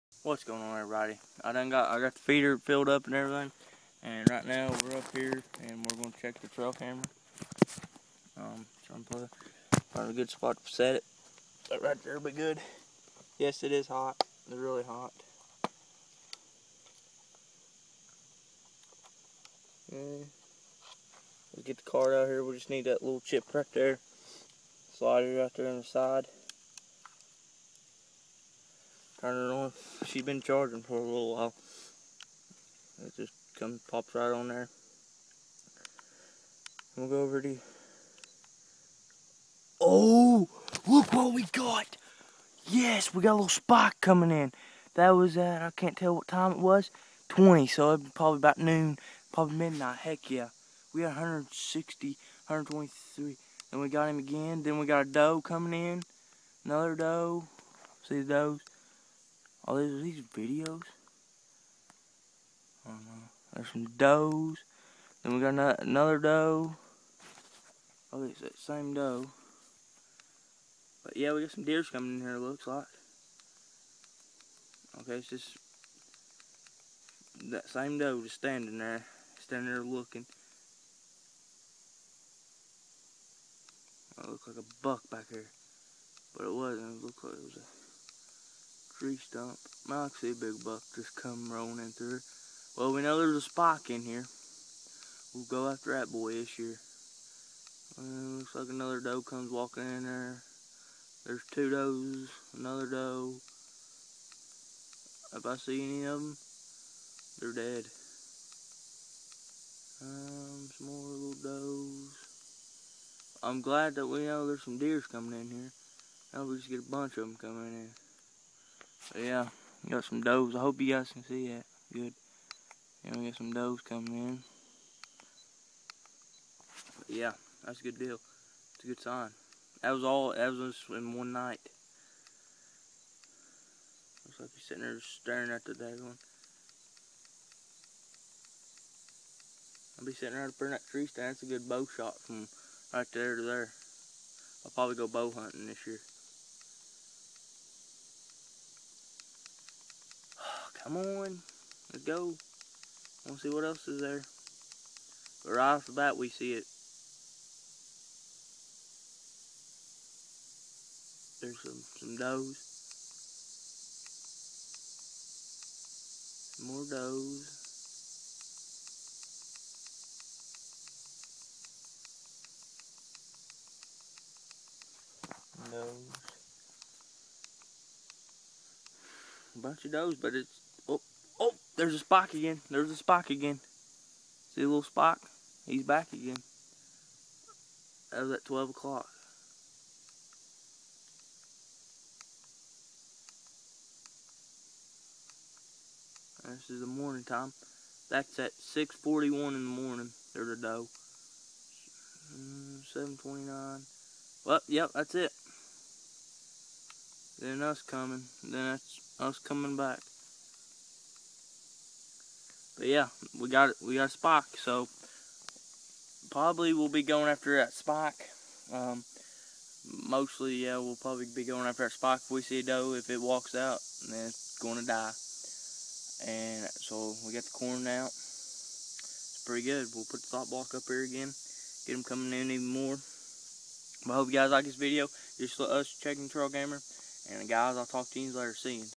cheaking the trail camera
knowed that we had some deer coming in